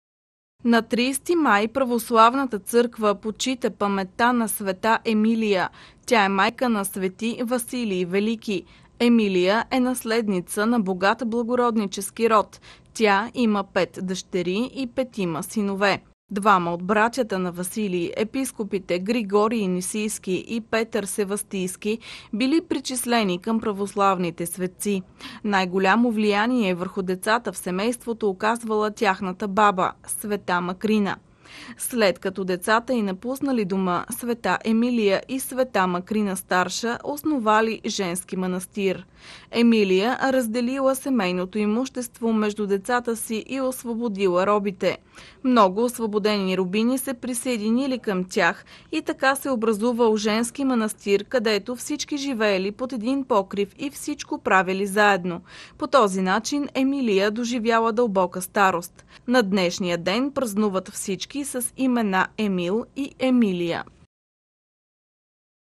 Почитаме паметта на Света Емилия /30.05.2018 г./
На 30 май Православната църква почита паметта на Света Емилия, майка на св. Василий Велики. Емилия е наследница на богат благороднически род, тя има пет дъщери и петима синове.
На този ден празнуват всички с имена Емил и Емилия